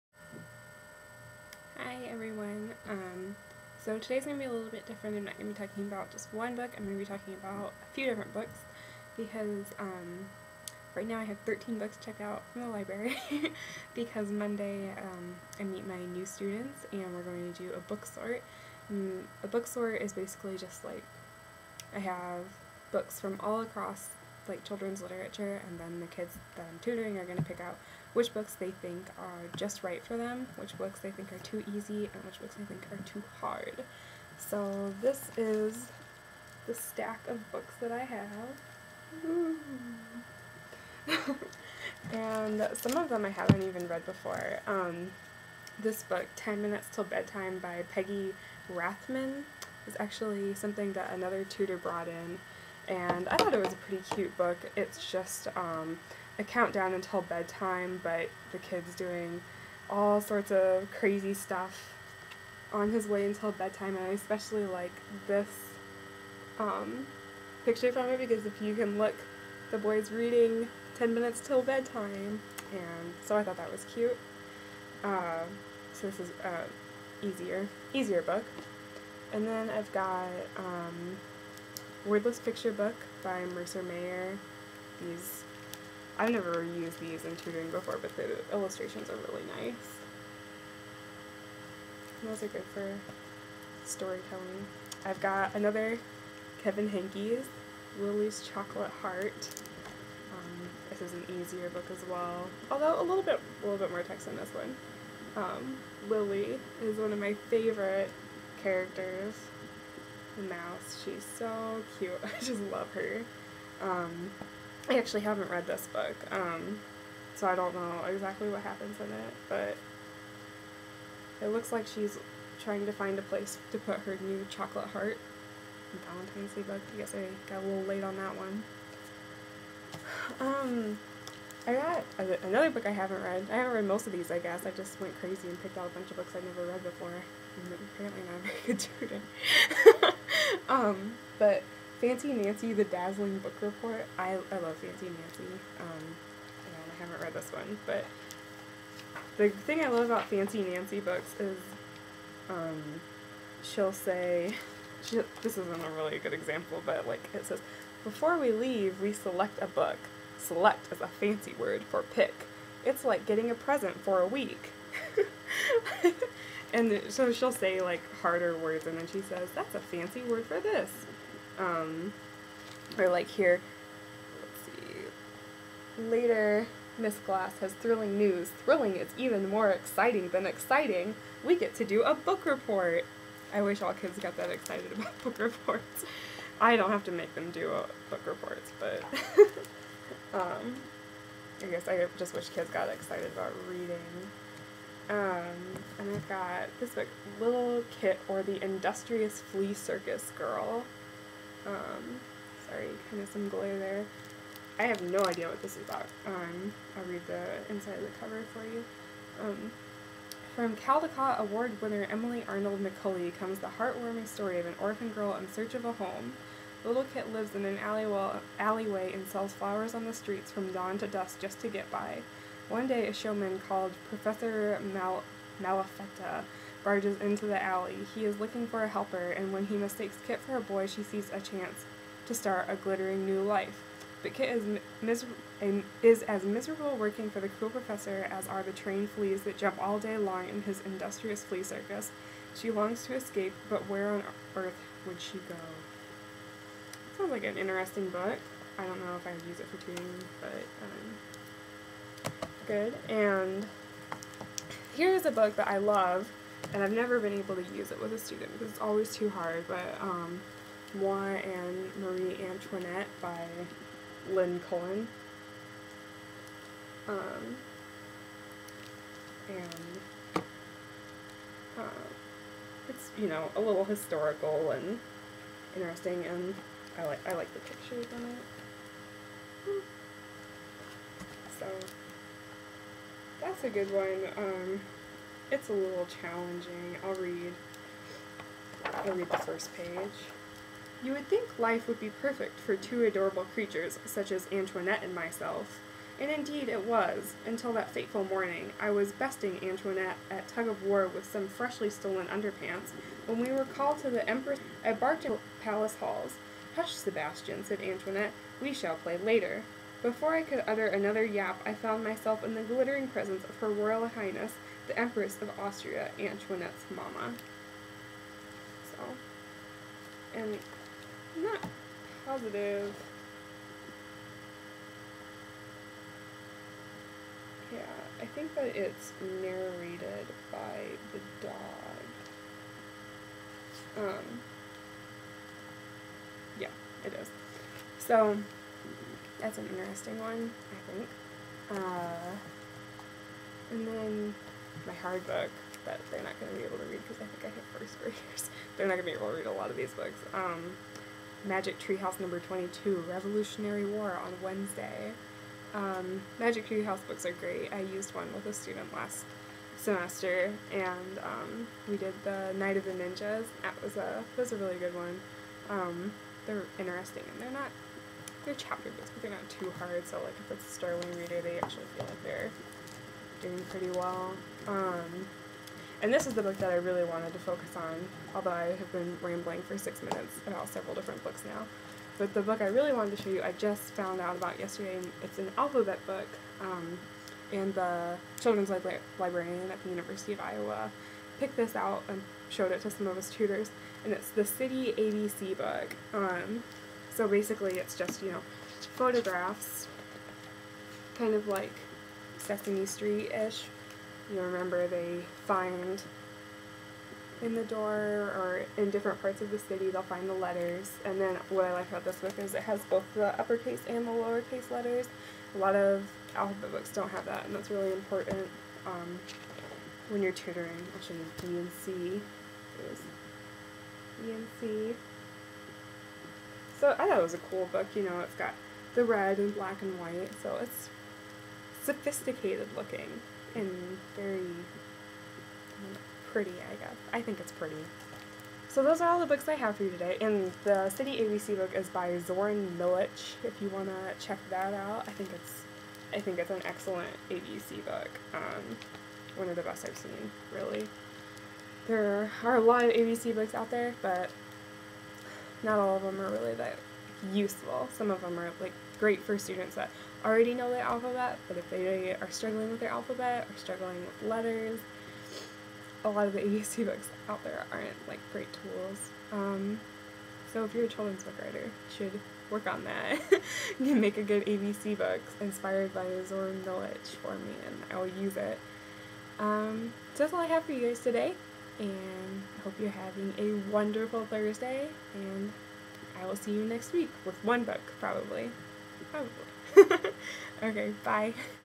Children's Book Thursday
Ash from English Major's Junk Food goes through several children's books across children's literature.